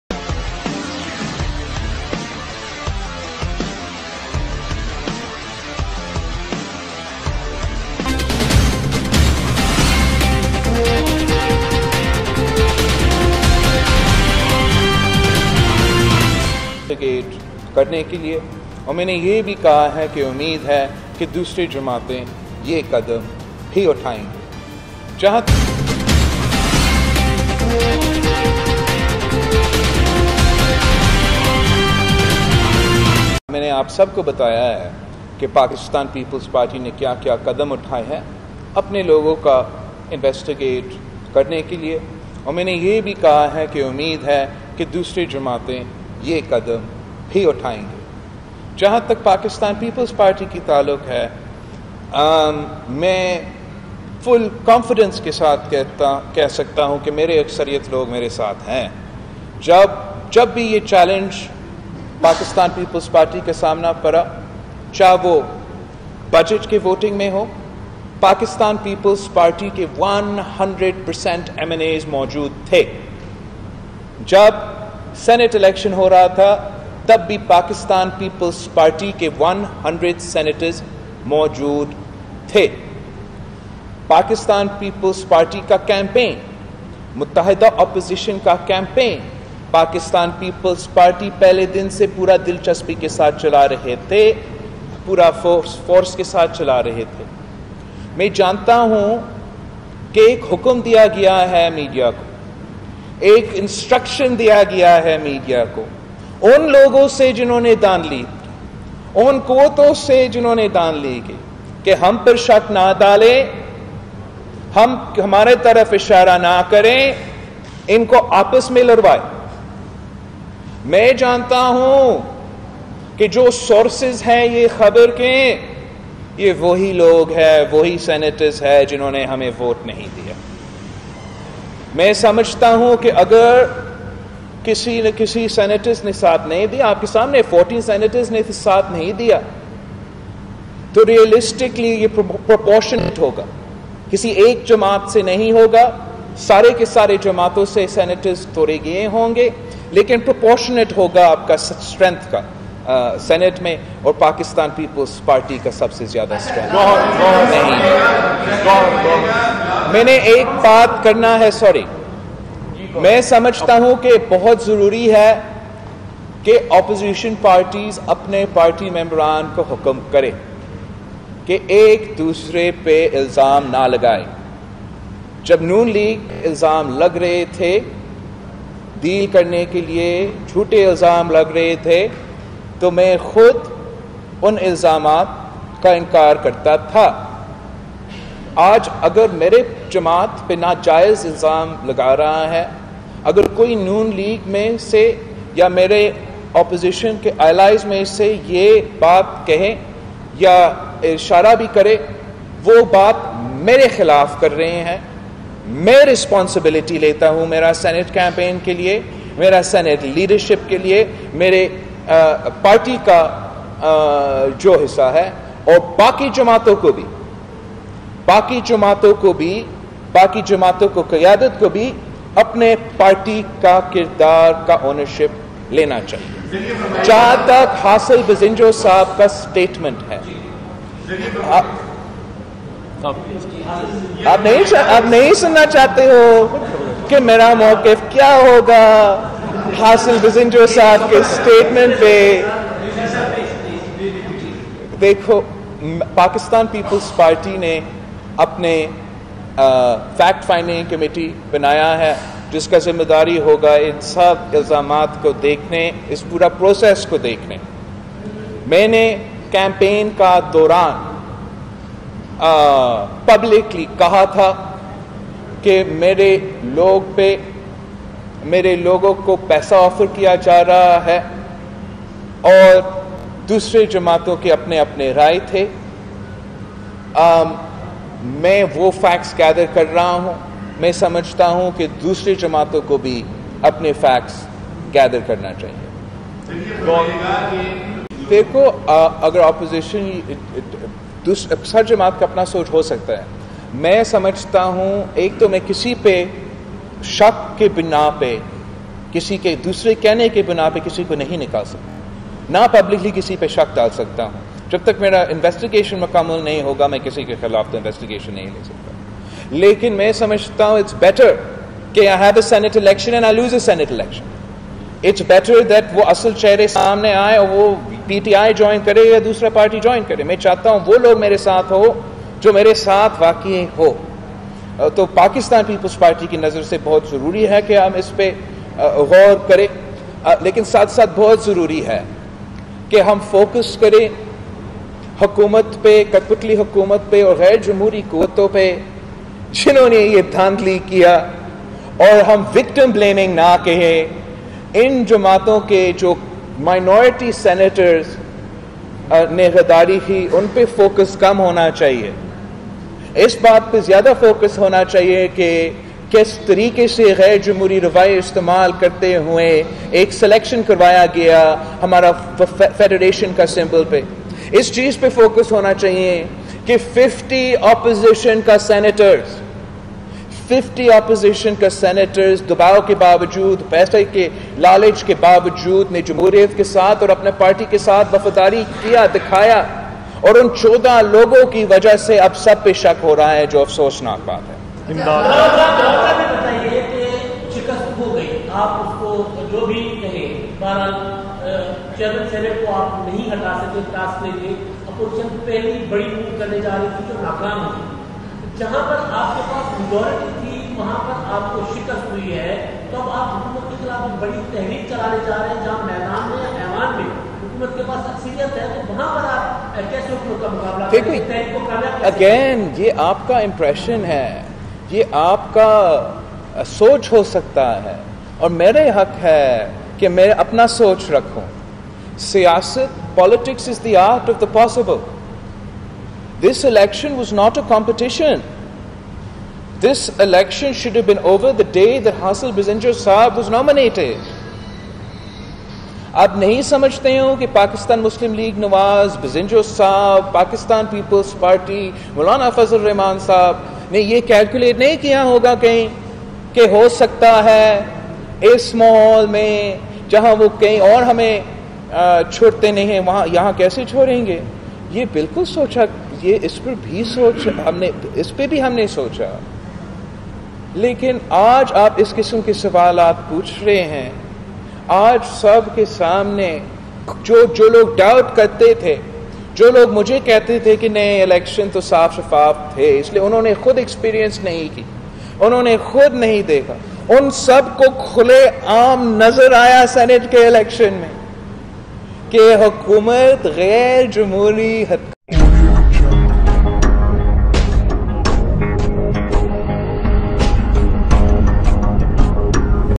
بلاول بھٹو زرداری نےکہا کہ میں سینٹ الیکشن میں اپنی پارٹی کے سینٹرز کے کردار کی ذمہ داری لیتا ہوں بل
میں سینٹ الیکشن میں اپنی پارٹی کے سینٹرز کے کردار کی ذمہ داری لیتا ہوں اور سمجھتا ہوں کہ دیگر پارٹیوں کو بھی اپنے اراکین کے کردار کی ذمہ داری لینی چاہیئے۔
چیئرمین پاکستان پیپلز پارٹی بلاول بھٹو زرداری